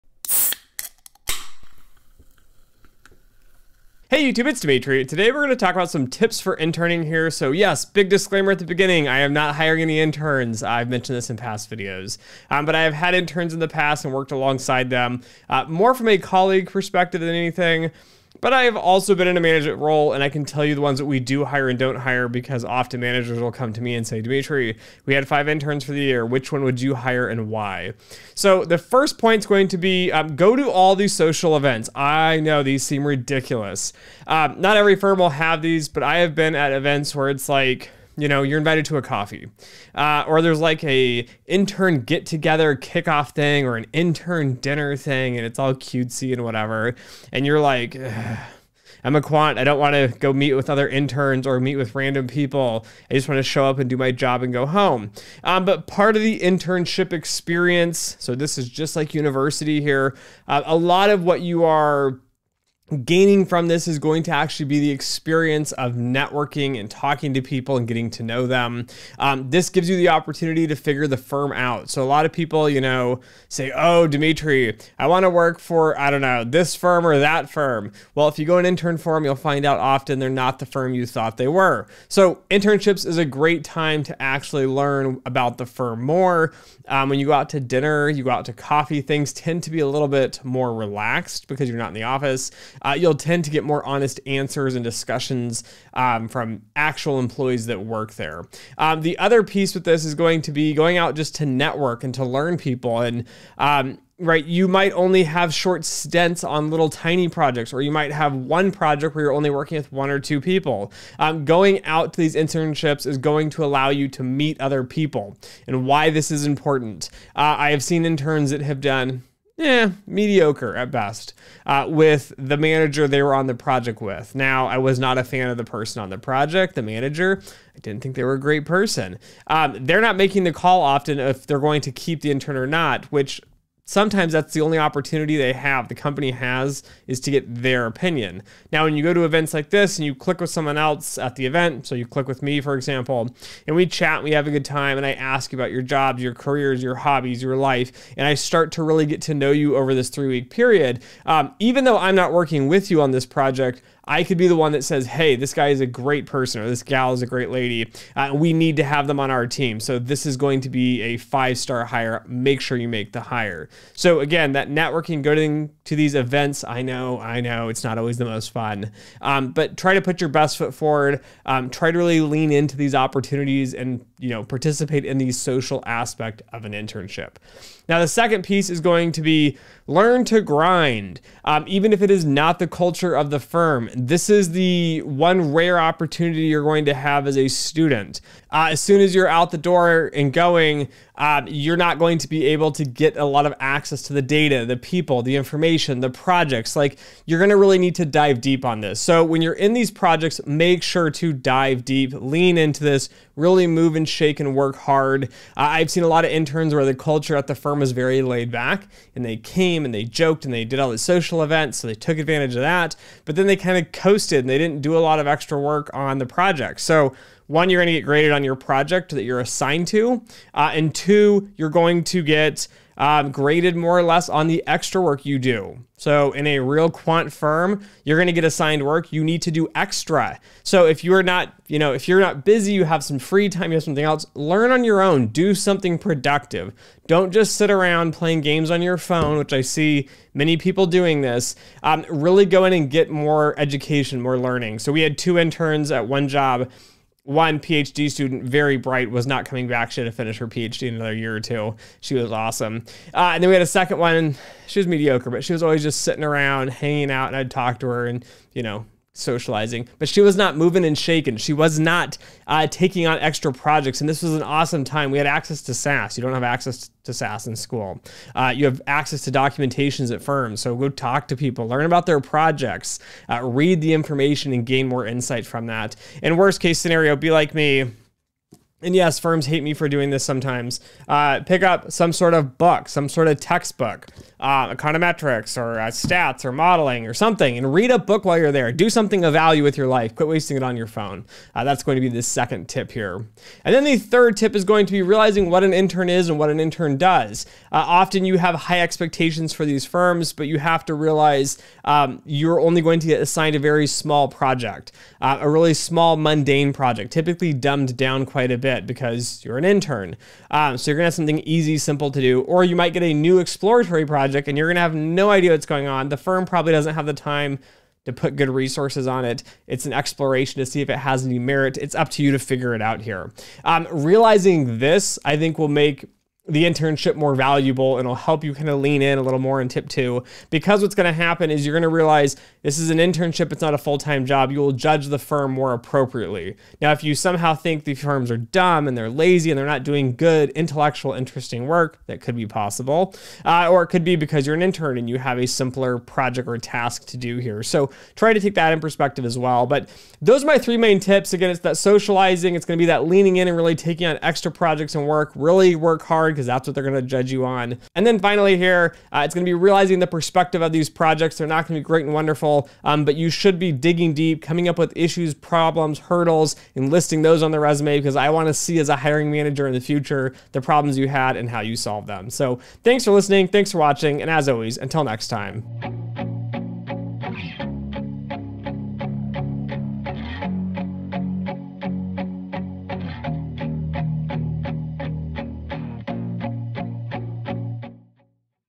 Maximizing Internship Experience
Internships are very hard to come by in the quant space! Don't waste your time as it will be over before you know it.

1) Socialize like crazy
2) Grind like no one is watching
3) Realize the internship is a simplified version of the real work